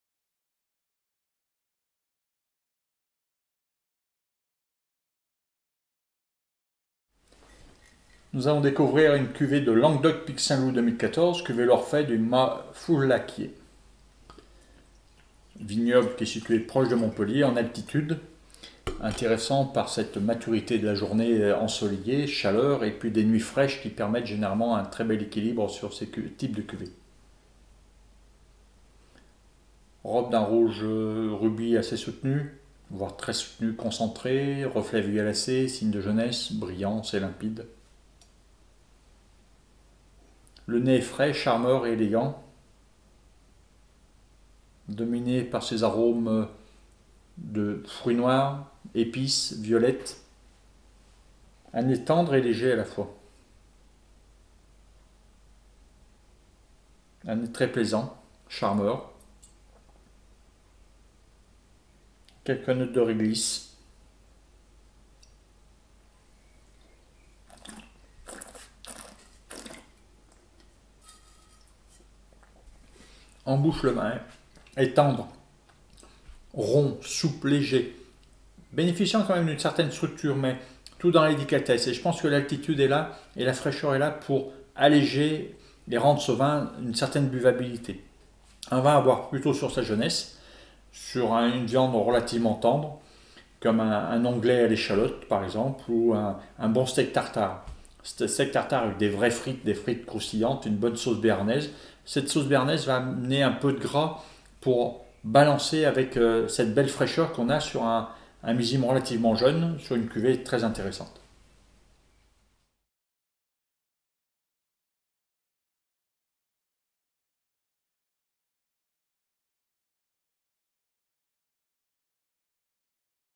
Pic Saint Loup 2014 Vin du Mas Foulaquier Dégustation par le Sommelier Arnaud Chambost
Le Sommelier Arnaud Chambost, Meilleur Ouvrier de France (MOF) Sommellerie 2000 déguste un vin rouge de l'appellation Languedoc Pic Saint Loup. Cette vidéo de dégustation du Languedoc Pic Saint Loup Cuvée l’Orphée millésime 2014 du Domaine Mas Foulaquier est réalisée par HoMetsVins (site internet sur l'accord mets vins - 1er réseau Click & Collect dédié au vin) en partenariat avec le
Ce vignoble d’altitude se situe proche de Montpellier. C’est une appellation intéressante à cause de l’ensoleillement de la journée et de la fraîcheur de la nuit qui va apporter un bel équilibre au vin.
La robe de ce Pic Saint Loup Cuvée l’Orphée 2014 du Mas Foulaquier est rubis soutenue avec des reflets violets. La robe est brillante et limpide.
Le nez de ce Pic Saint Loup Cuvée l’Orphée 2014 du Mas Foulaquier est marqué par des arômes de fruits noirs et de violette. On retrouve une touche d’épices. Le nez de ce Pic Saint Loup Cuvée l’Orphée 2014 du Mas Foulaquier est élégant et charmeur. On retrouve également des notes de réglisse dans ce vin du Languedoc.
A la dégustation, la bouche de ce Pic Saint Loup Cuvée l’Orphée 2014 du Mas Foulaquier est tendre avec une belle souplesse. C’est un vin qui a une certaine structure mais une belle légèreté. La bouche est délicate.
Vous apprécierez ce Pic Saint Loup Cuvée l’Orphée 2014 du Mas Foulaquier dès maintenant pour accompagner un onglet de bœuf à l’échalote ou un steak tartare servi avec une sauce béarnaise.
Ce Pic Saint Loup Cuvée l’Orphée 2014 du Mas Foulaquier est une cuvée très intéressante.